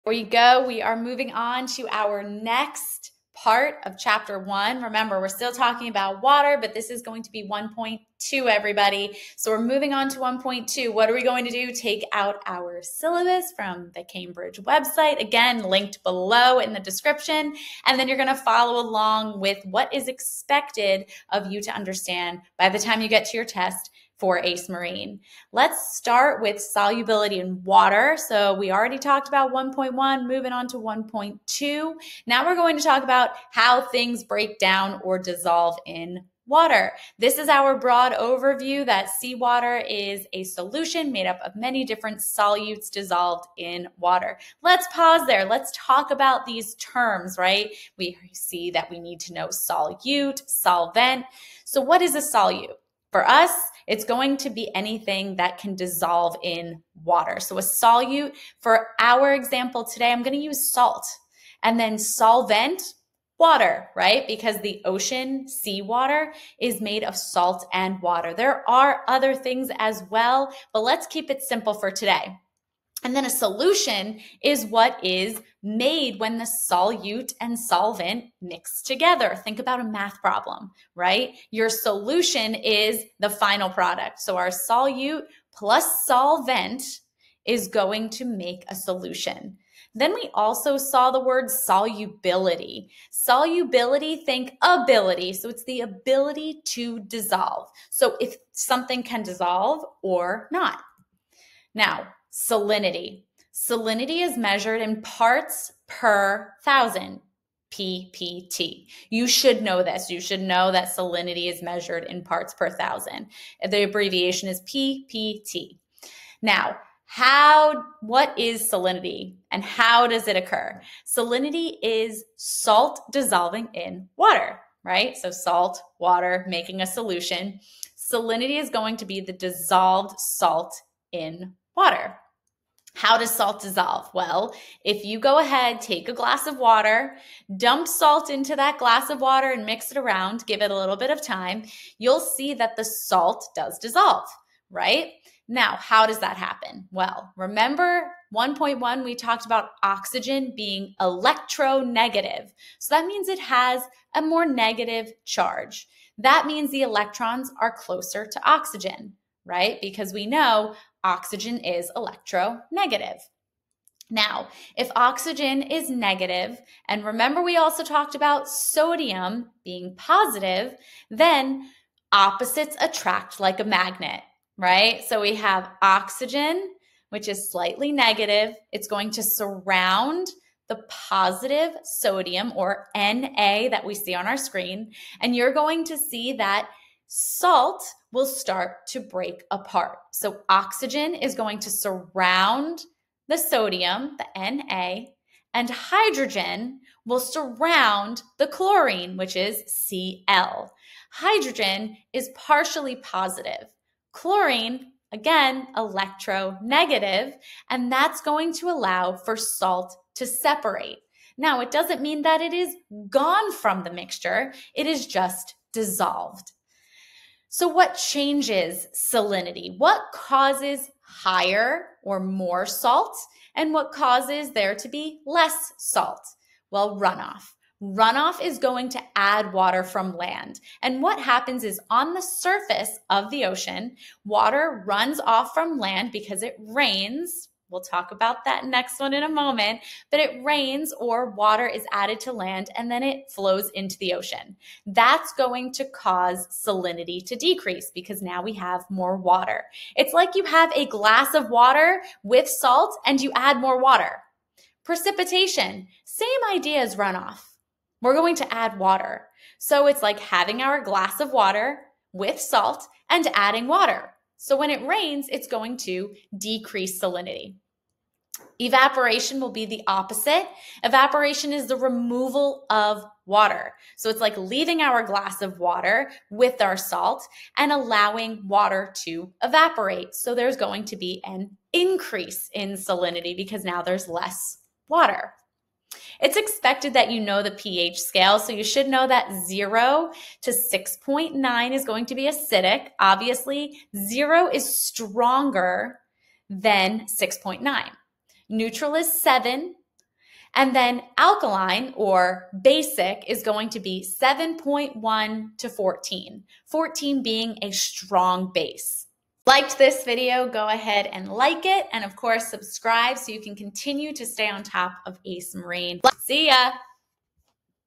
AICE Marine Science Understanding Water Chemistry 🌊
In this video, I guide you through the basics of water chemistry, focusing on solubility, salinity, and pH levels. I explain how different factors like runoff, precipitation, and evaporation affect salinity levels in water. By the end, you'll have a clear understanding of how these concepts interplay in marine environments, preparing you for the AICE Marine test.